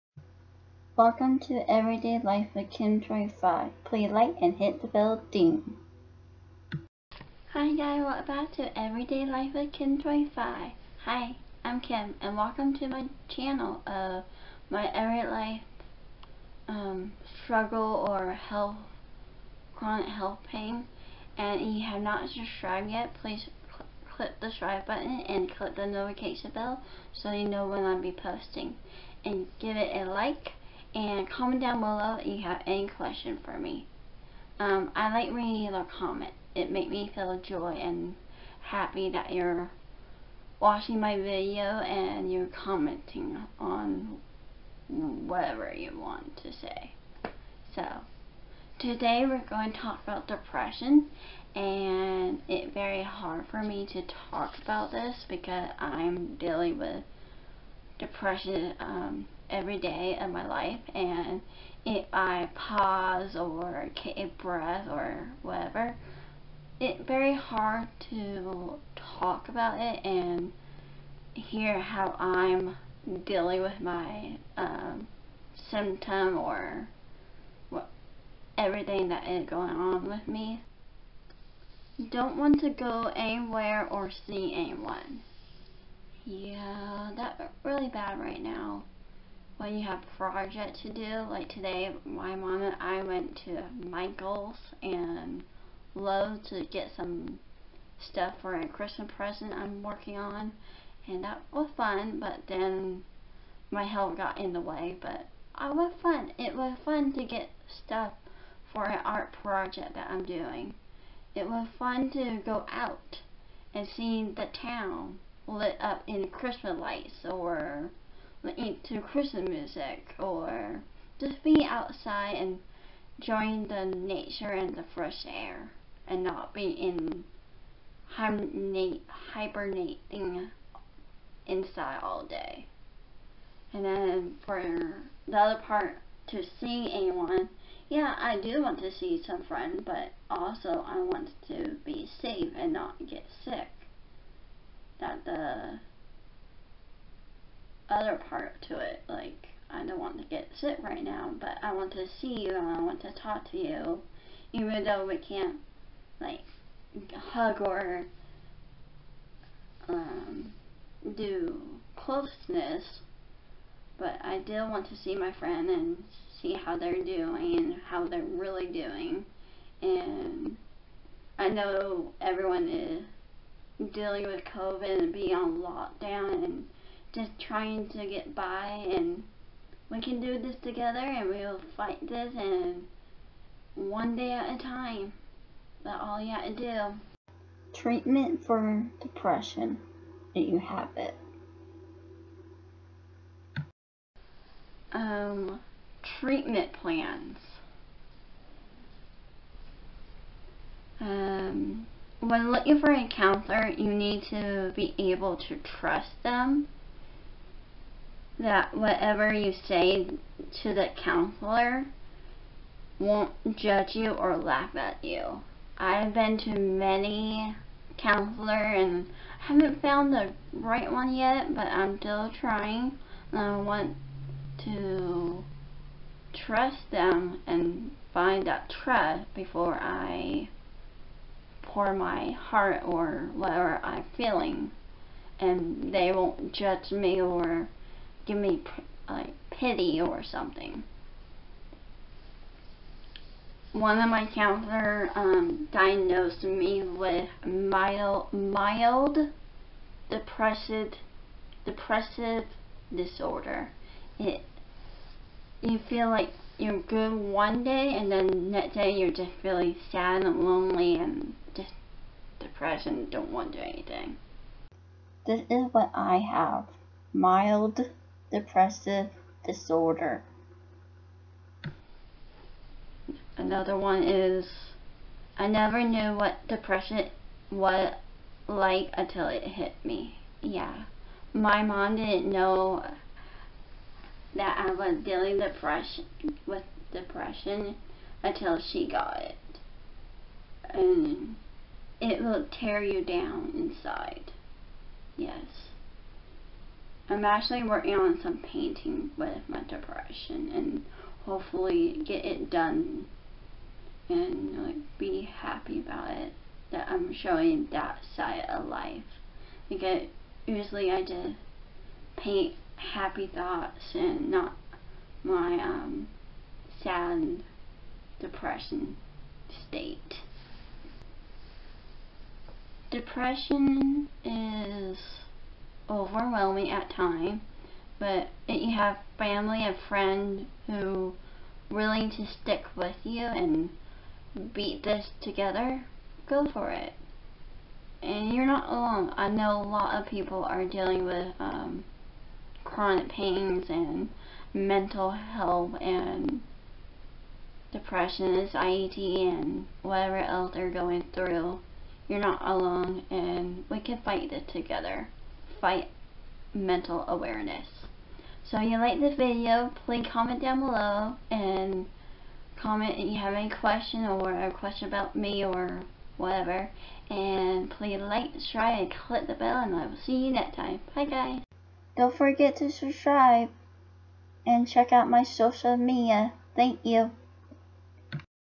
Everyday Life with Kim 25: How do I cope with Depression?
How do I cope with Depression, by doing something about it. Talking about my Mental Health Awareness struggles that I am experiencing with them and letting you guys knows how hard it is.

the comment section. Thank you.